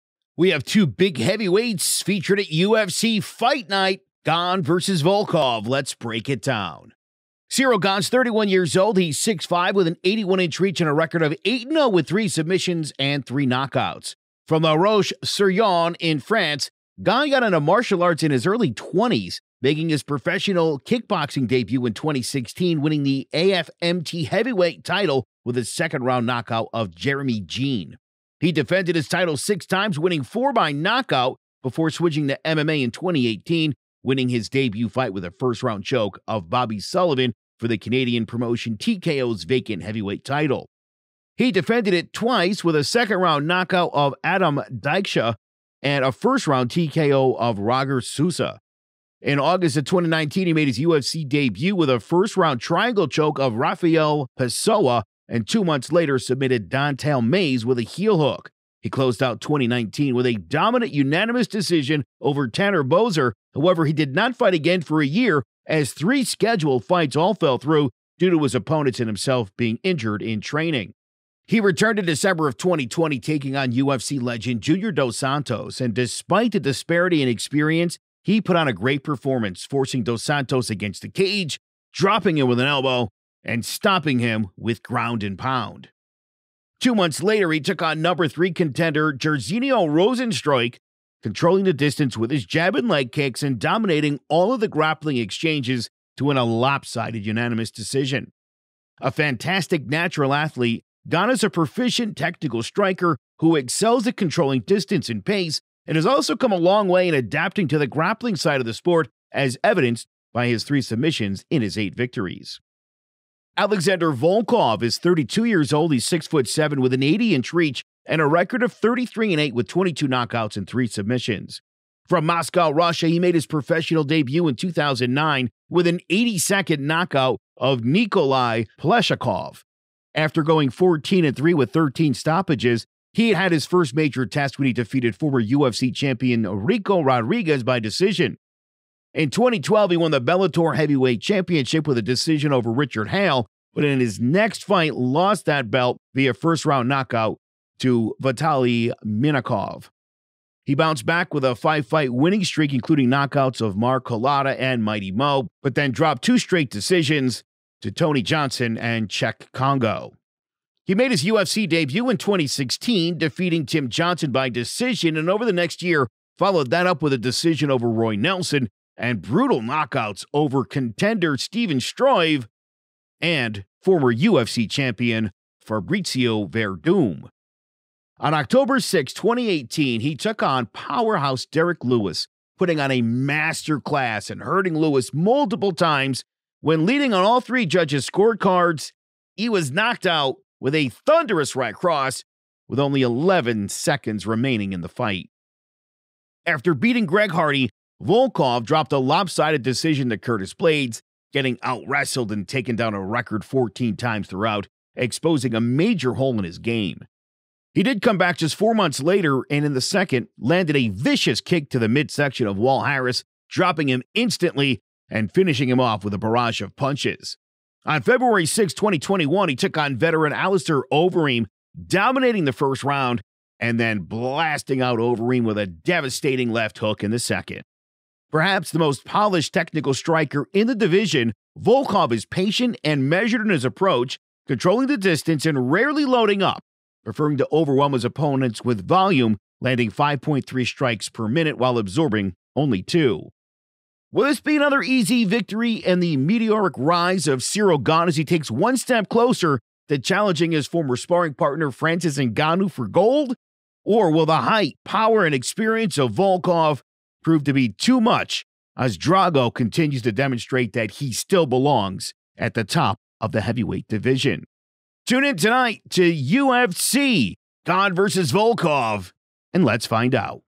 Gane vs Volkov Tale of the Tape
We have two big heavyweights at UFC Fight Night: Gane vs Volkov. Let's Break it Down.
Ciryl Gane 31 years old he’s 6’5” with an 81” reach and a record of 8-0 with 3 knockouts and 3 submissions. From La Roche sur Yon in France Gane got into martial arts in his early twenties making his professional kickboxing debut in 2016 winning the AFMT heavyweight title with a second round knockout of Jeremy Jeanne. He defended his title 6 times winning 4 by knockout before switching to MMA in 2018 winning his debut fight with a first round front choke of Bobby Sullivan for the Canadian promotion TKO’s vacant heavyweight title. He defended it twice with a second round knockout of Adam Dike-shah  and a first round tko of Roggers Souza. In august of 2019 he made his UFC debut with a first round triangle choke of Raphael Pessoa and two months later submitted Dontale Mayes with a heel hook. He closed out 2019 with a dominant unanimous decision over Tanner Boser however he did not fight again for a year as 3 scheduled fights fell through due to his opponents and himself getting injured in training. He returned in December of 2020 taking on UFC legend Junior Dos Santos and despite the disparity in experience he put on a great performance forcing Dos Santos against the cage dropping him with an elbow and stopping him with ground and pound. Two months later he took on number 3 contender Jairzinho Rosenstruik controlling the distance with his jab and leg kicks and dominating all of the grappling exchanges to win a lopsided unanimous decision. A fantastic natural athlete Gane is a proficient technical striker who excels at controlling distance and pace and has also come a long way in adapting to the grappling side of the sport as evidenced by his 3 submissions in his 8 victories.

Alexander Volkov 32 years old he’s 6’7” with an 80” reach and a record of 33-8 with 22 knockouts and 3 submissions. From Moscow Russia he made his professional debut in 2009 with an 80 second knockout of Nikolai Pleshakov. After going 14-3 with 13 stoppages he had his first major test when he defeated former UFC champion Ricco Rodriguez by decision. In 2012 he won the Bellator heavyweight championship with a decision over Richard Hale but in his next fight lost his belt via first round knockout to Vitaly Min-a-kov. He bounced back with a 5 fight winning streak including knockouts of Mark Holata and Mighty Mo but then dropped two straight decisions to Tony Johnson and Cheick Kongo. He made his UFC debut in 2016 defeating Tim Johnson by decision and over the next year followed that up with a decision over Roy Nelson and brutal knockouts over contender Stefan Struve and former UFC champion Fabricio Werdum. On October.6,2018 he took on powerhouse Derrick Lewis putting on a masterclass and hurting Lewis multiple times when leading on all 3 judges scorecards he was knocked out with a thunderous right cross with only 11 seconds remaining. After beating Greg Hardy volkov dropped a lopsided decision to Curtis Blaydes, getting  out wrestled and taken down a record 14 times throughout, exposing a major hole in his game. He came back just 4 months later and in the second landed a vicious kick to the midsection of Walt Harris, dropping him instantly and finishing him with a barrage of punches. On February.6, 2021 he took on veteran Alistair Overeem dominating the first round and then blasting out Overeem with a devastating left hook in the second. Perhaps the most polished technical striker in the division, Volkov is patient and measured in his approach, controlling the distance and rarely loading up, preferring to overwhelm his opponents with volume landing 5.3 strikes per minute while absorbing only 2.
Will this be another easy victory in the meteoric rise of Ciryl Gane as he takes one step closer to challenging former sparring partner Francis N’gannou for gold? Or will the height, power, and experience of Volkov prove to be too much too soon as Drago continues to demonstrate that he still belongs at the top of the division?

Tune in and let's find out.